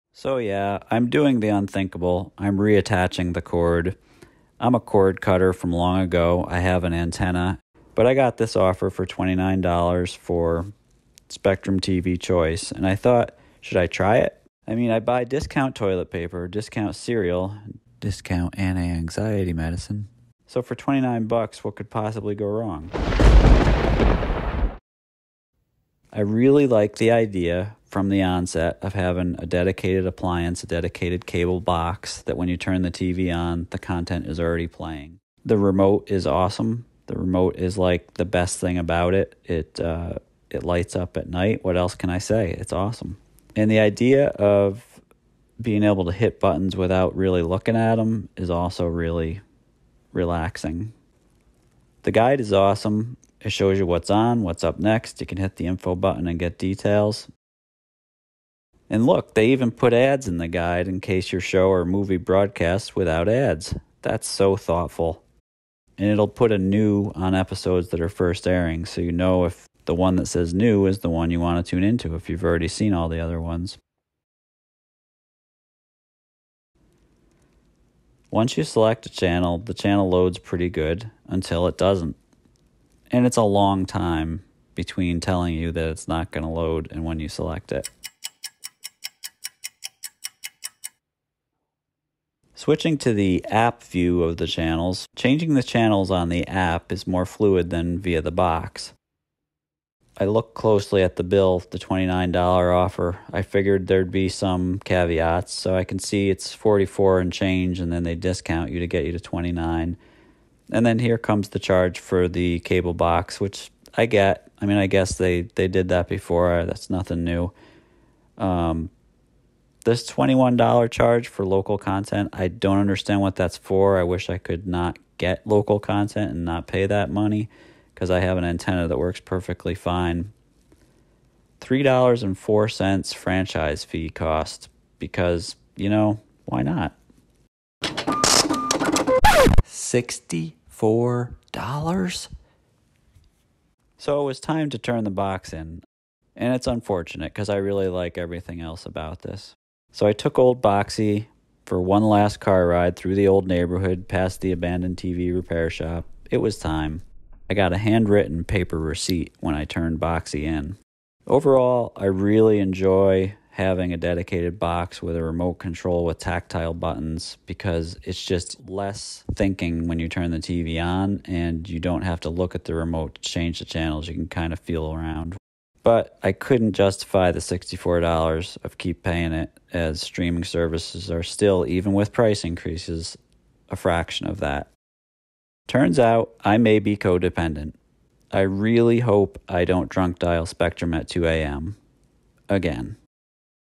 Spectrum TV Choice FULL Costs Review
From the offer price (*with it's own asterisk) on the original mailer to the actual dollars paid:
what is Spectrum TV Choice, what does it look like and most importantly, what does it ACTUALLY cost??

Cutting the Cord?
Antennas are a good place to start, and Antennas Direct has a pretty good range of products: